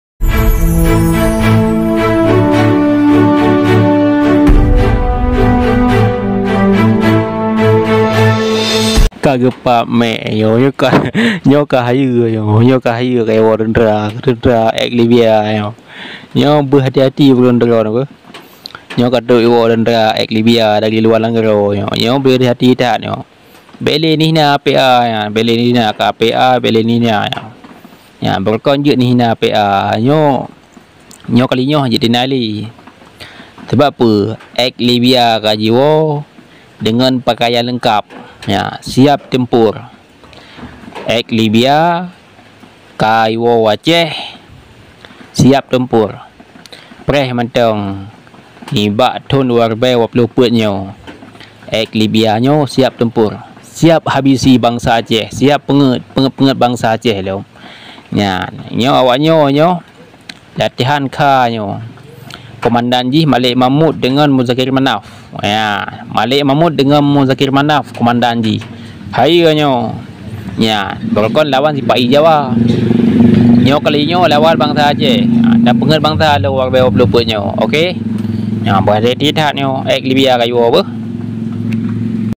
Gam Elit Libiya Pulang Siap Perang | Gam Luar Nanggroe Pulang Ke Aceh
Pasukan Gam Luar Nanggroe Pulang Ke Aceh Siap Tempur 2024.